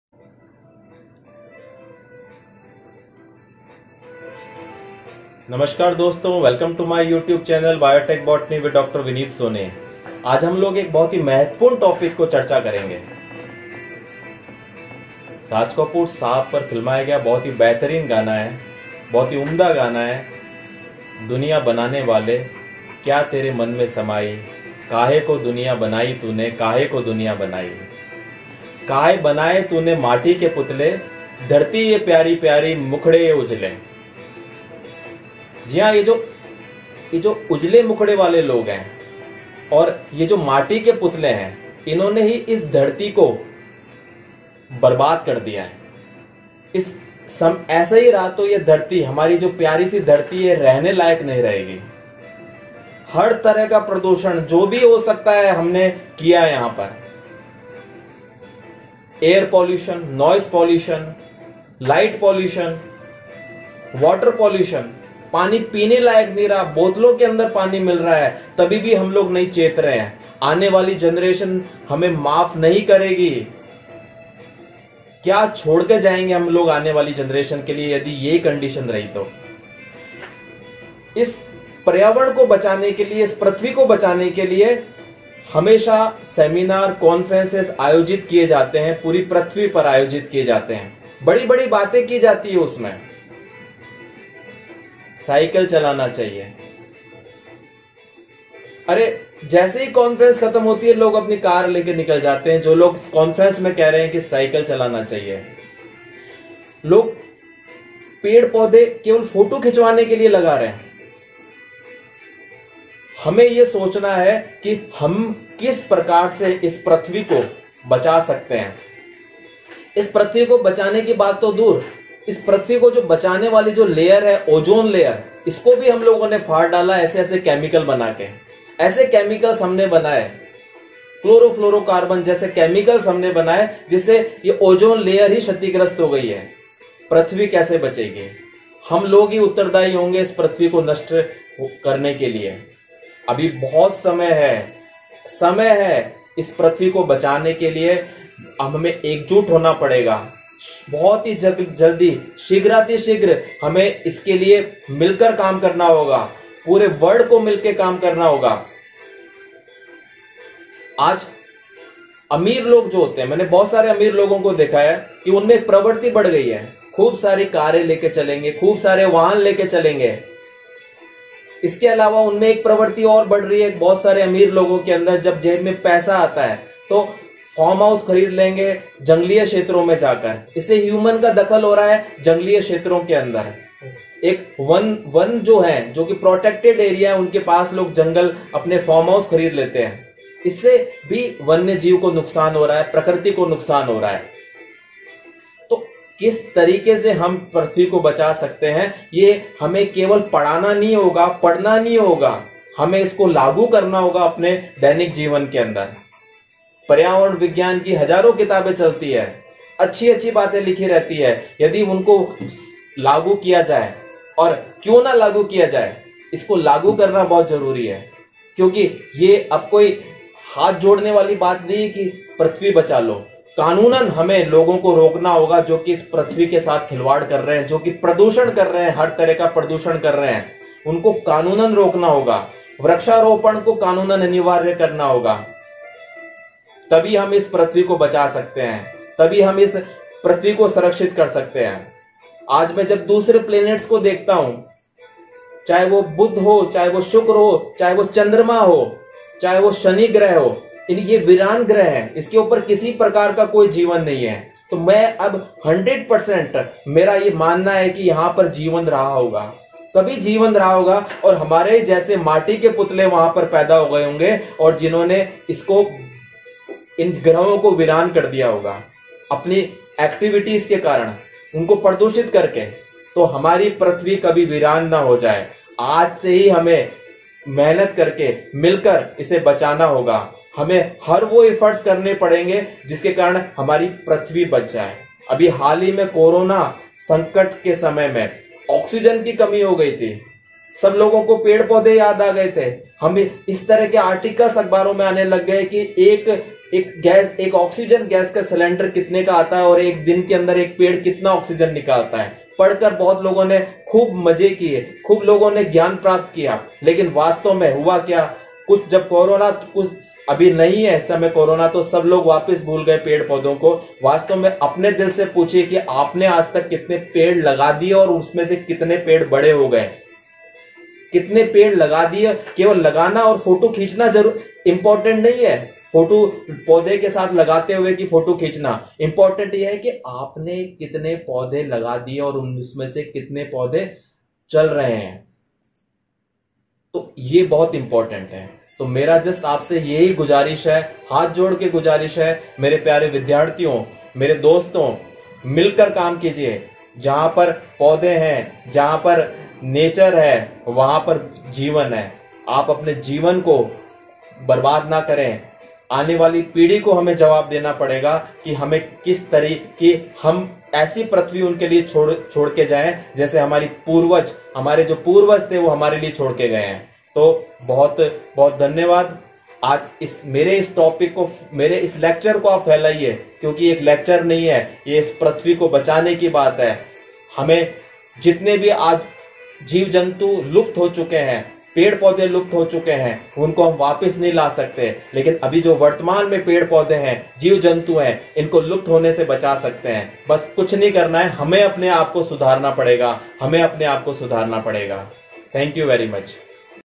| Save our Planet | पृथ्वी बचाओ | A Lecture by Dr Vineet Soni |
Hello Students, In this video I will be discussing about "Saving our planet from pollution".

Dr. Vineet Soni
MSc, PhD, PostDoc (Switzerland), FMA, FBS, FLS
Plant Biotechnology and Bioenergetics Laboratory
Department of Botany, Mohanlal Sukhadia University
Udaipur, Rajasthan, India